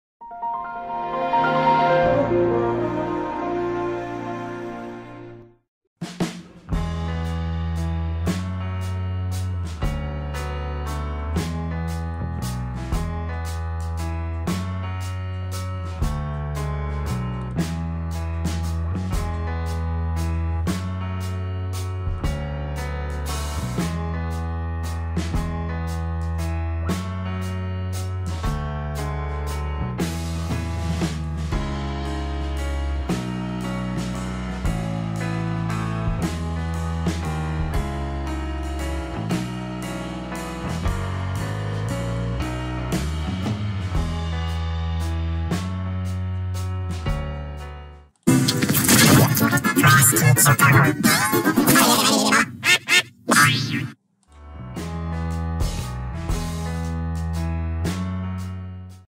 How To Make Yappy Effect On Sony Vegas Pro
Made on Wednesday, May 1, 2019, 3:19:31 to today on 3:20:16 pm

Yes, it's actually called Yappy X (Yappy Effect)

Former Title: Siberiaball Productions' Happy Effect

This is Siberiaball Productions' version of the Happy Effect (Happy X) on Sony Vegas Pro. It has the same pitch set as G-Major 20.

Upload Time: May 9, 2019, 12:44 am